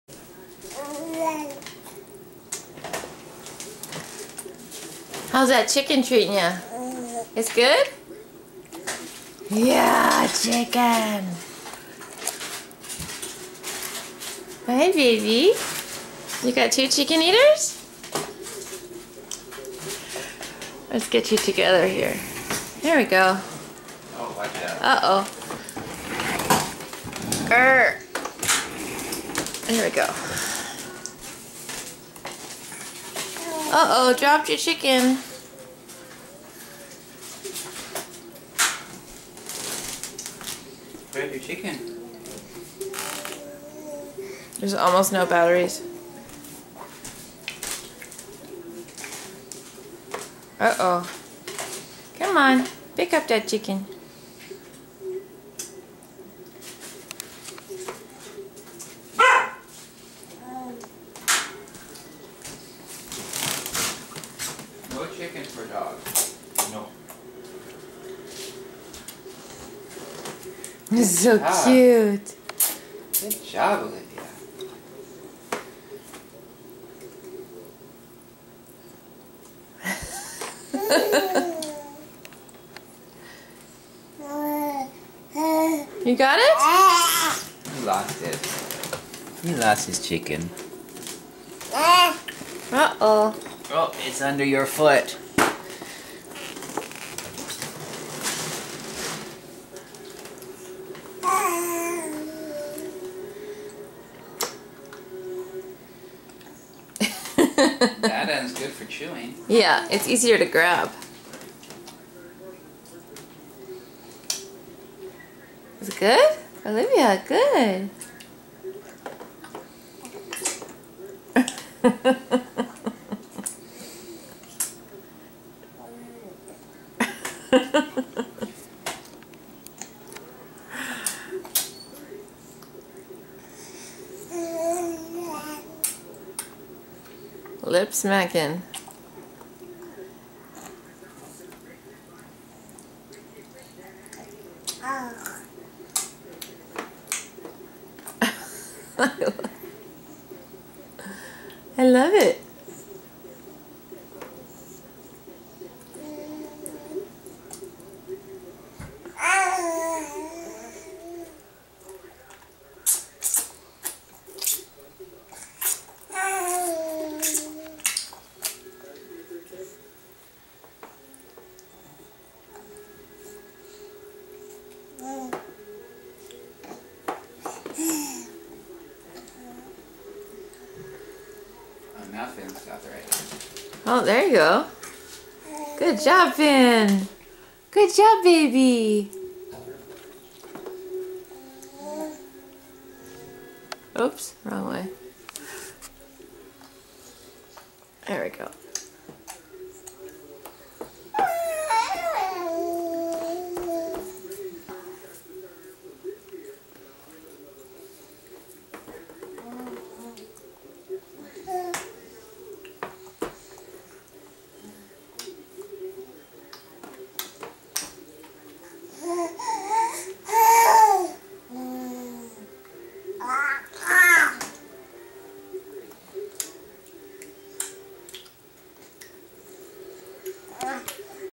drumstick babies
long video involving two incredibly cute babies eating chicken drumsticks